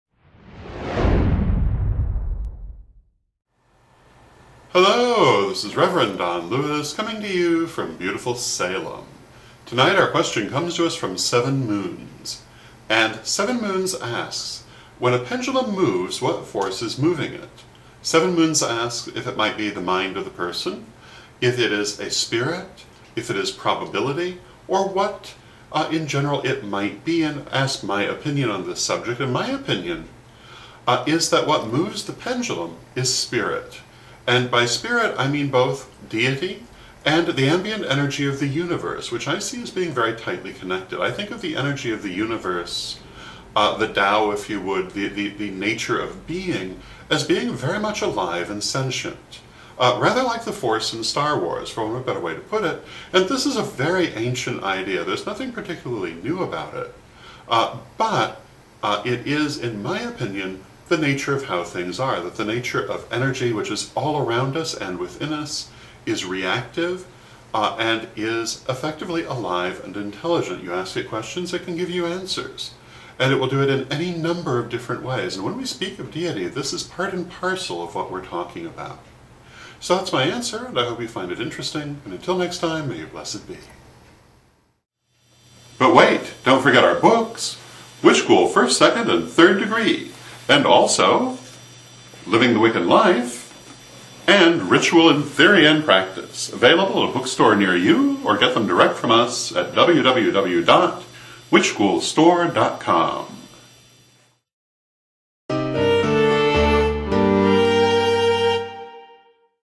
Rev. Don's Vlog - Penultimate Pendulum
Rev. Don discusses what makes a pendulum move, in response to a question from SevenM00ns. See all of Rev. Become a Supporting Member of Witch School just $29.99 until Yule